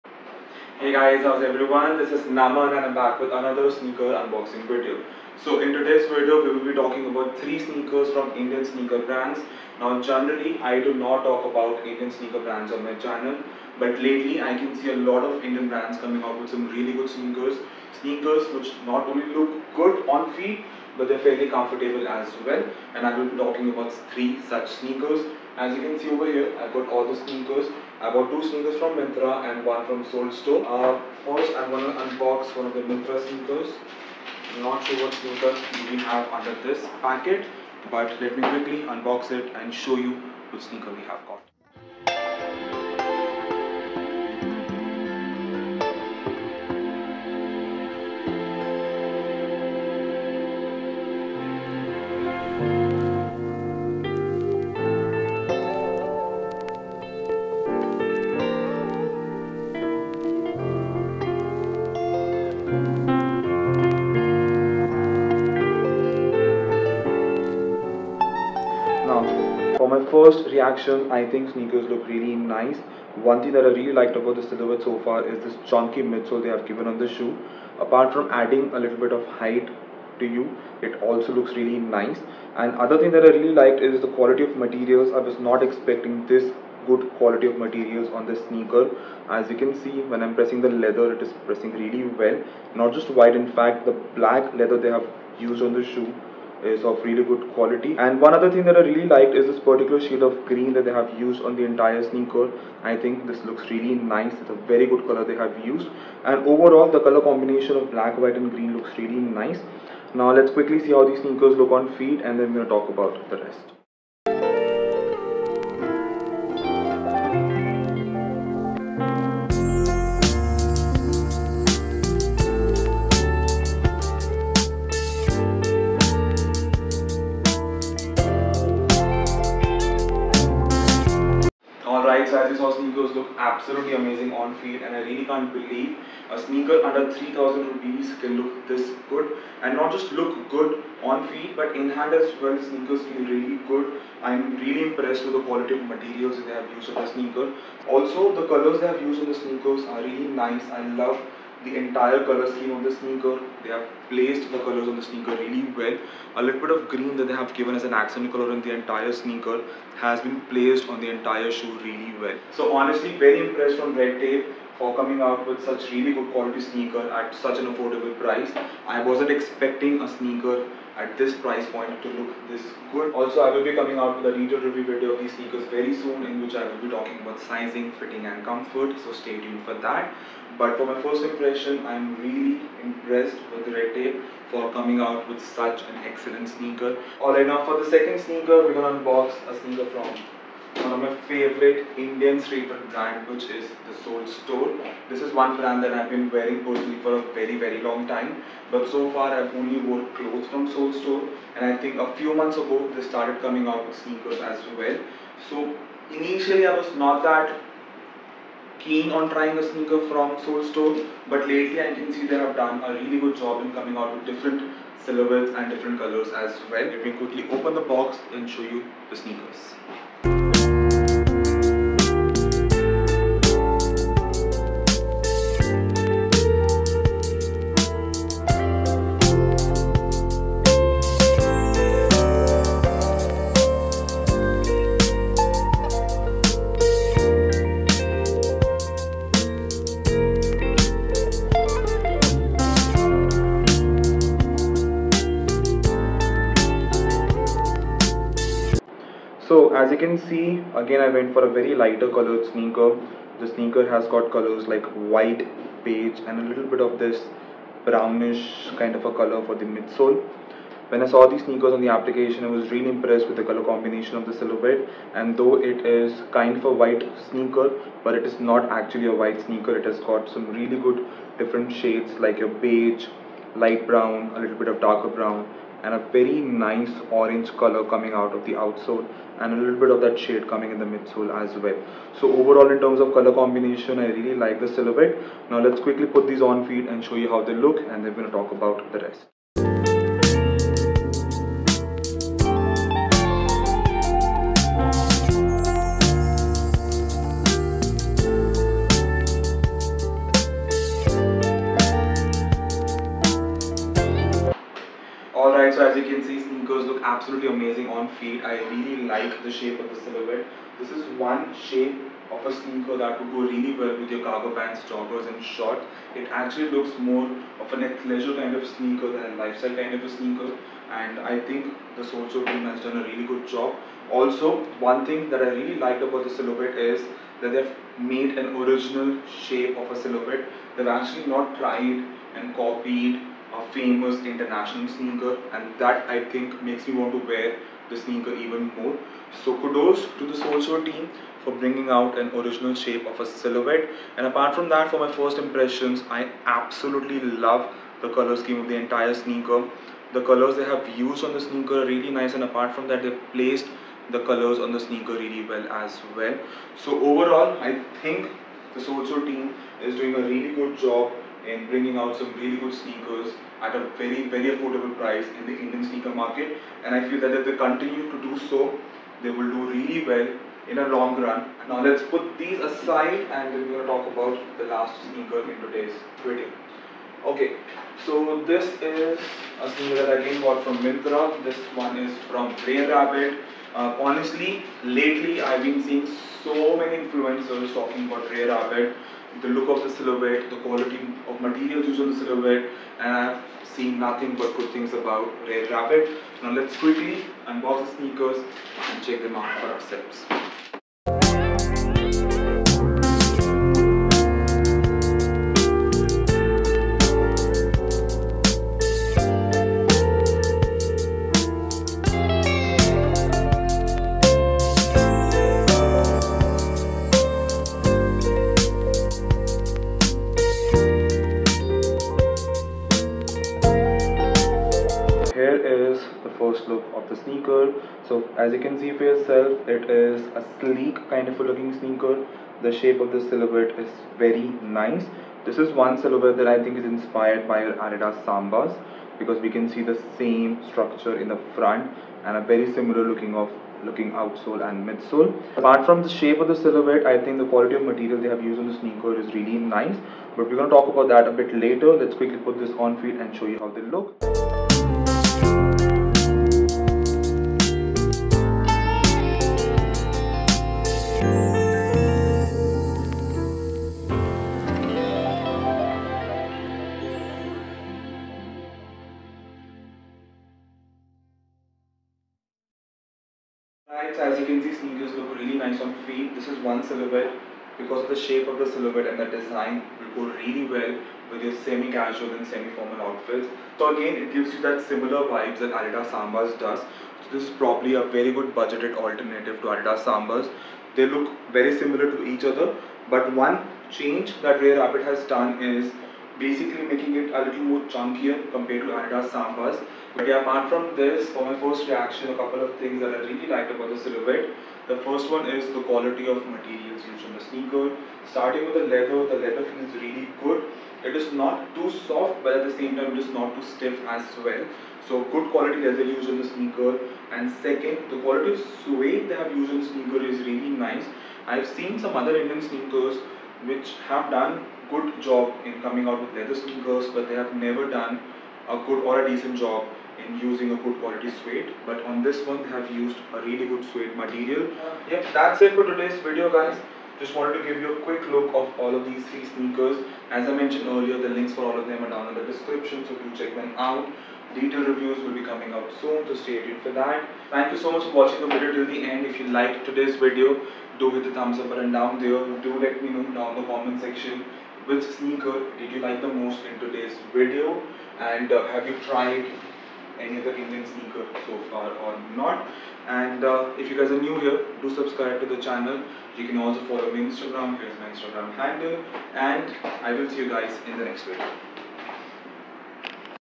Trying sneakers from Indian brands | Rare Rabbit, The Souled store & Redtape
Hey, in this video I have talked about the top 3 sneakers from Indian brands.